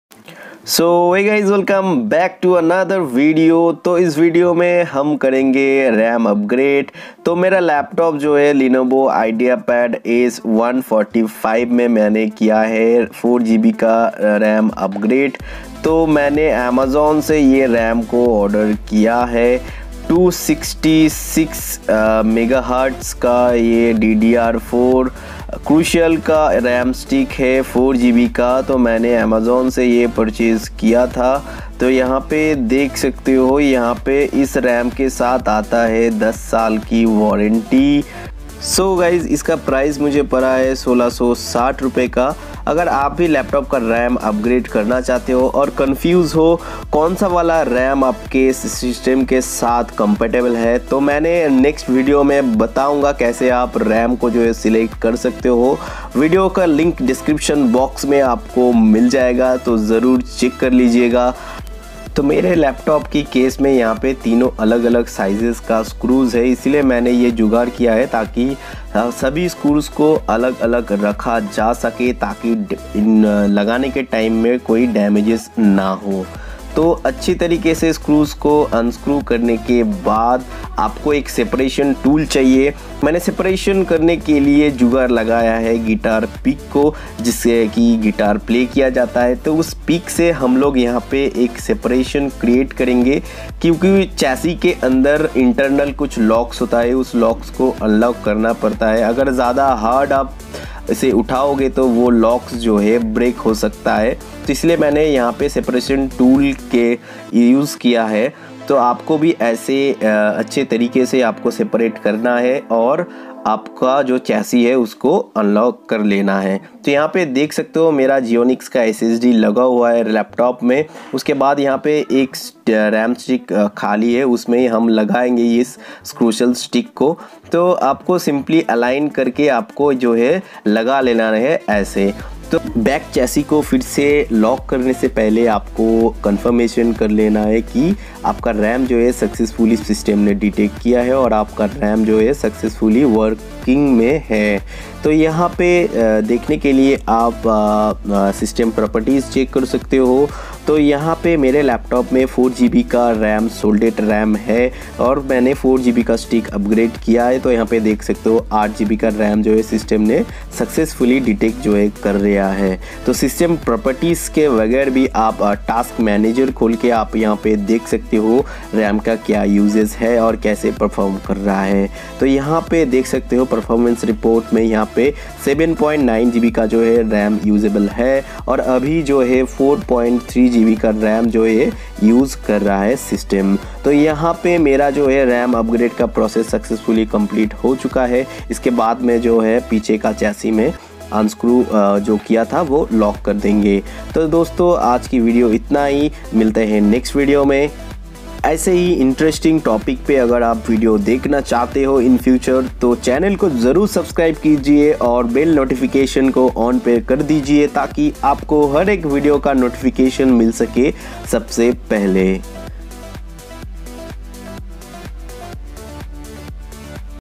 RAM Upgrade Lenovo Ideapad s145 i3 8th gen || 2133 Mhz 4 GB DDR4 Model-81VD0082IN Type 81VD
Processor: 8th Gen Intel Core i3-8130U processor, 2.2GHz base processor speed, 3.4 GHz Max speed, Dual cores, 4MB SmartCache
Operating System: Pre-loaded Windows 10 Home with lifetime validity
Display: 15.6-inch screen with (1920X1080) full HD display | Anti Glare technology

Lenovo Ideapad s145 compatible RAM
Crucial 4GB DDR4 RAM

⏺️My Mic
⏺️My Tripod
⏺️My Smartphone
⏺️My Laptop
⏺️My Headphone
⏺️Y2 Tempered Glass